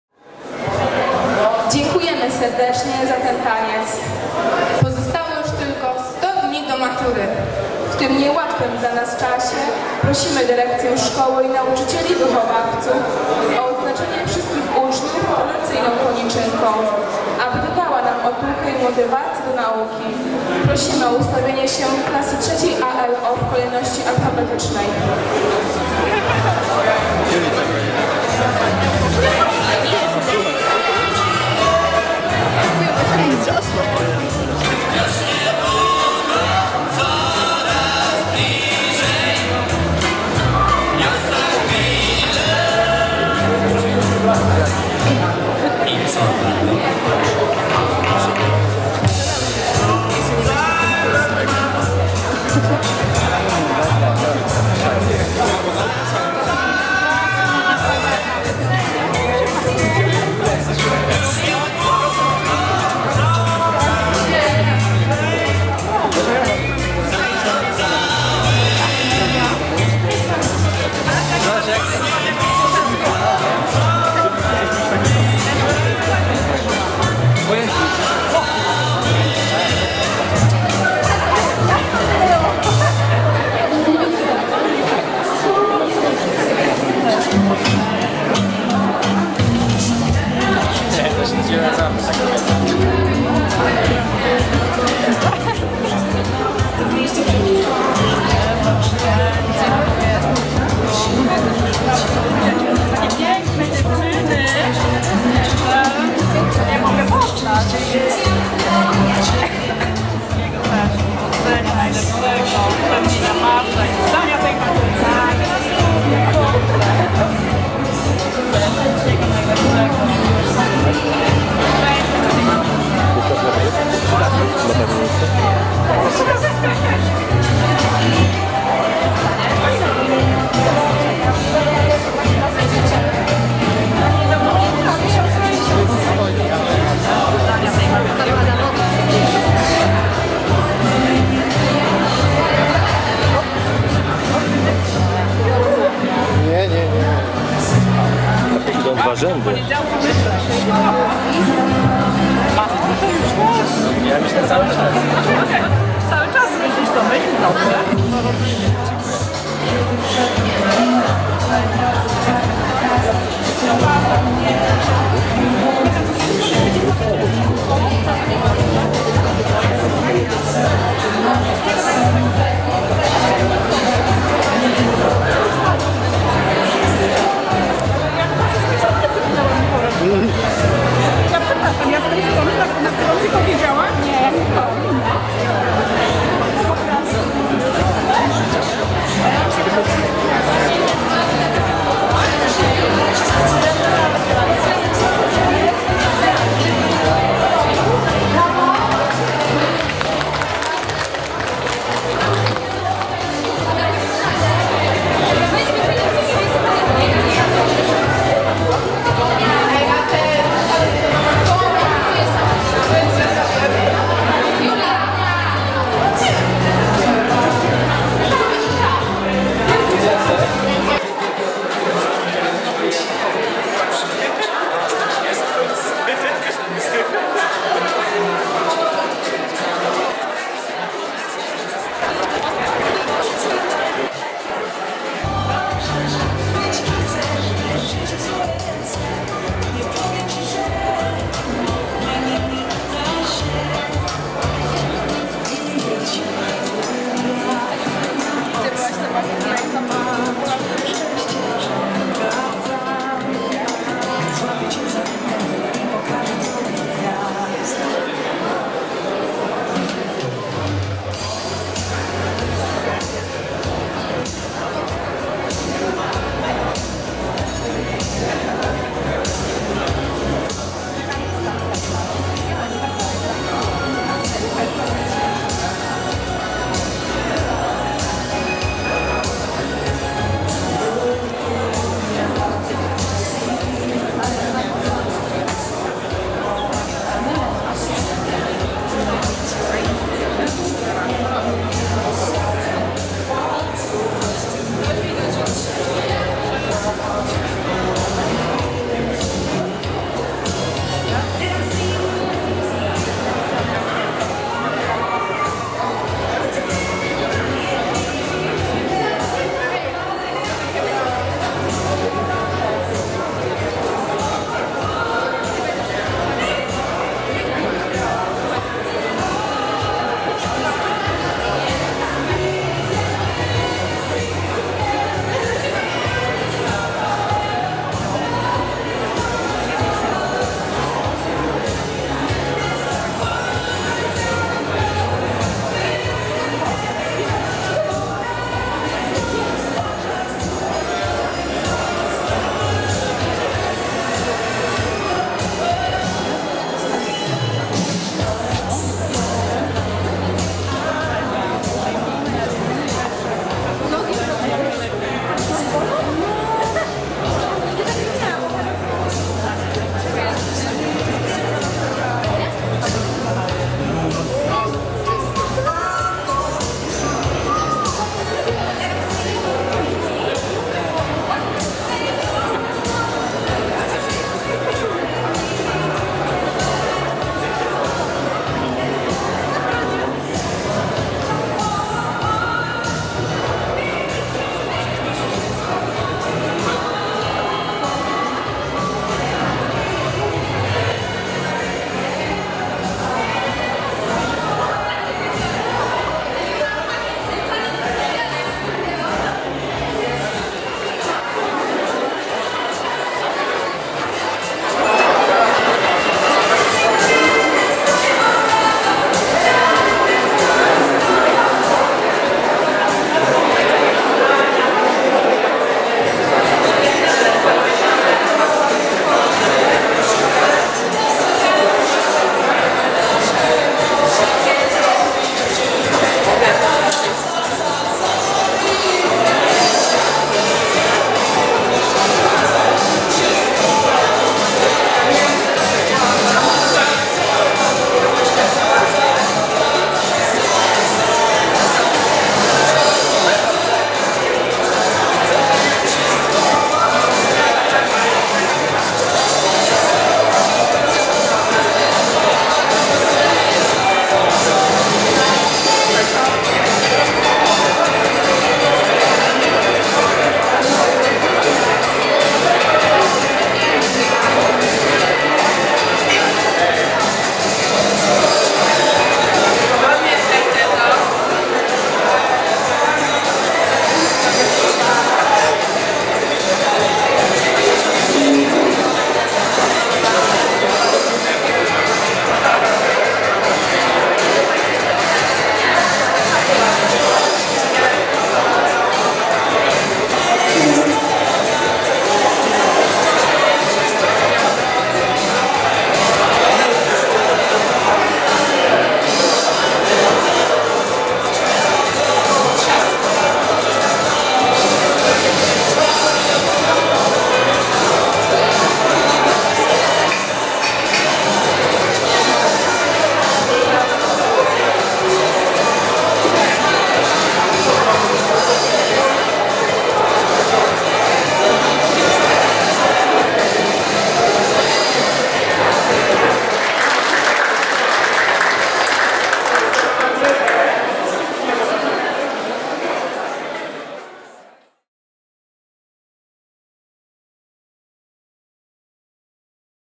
Studniówka: koniczynki
Tradycyjne przypinanie koniczynek na studniwce ZSLG Rataje 13 stycznia 2017r.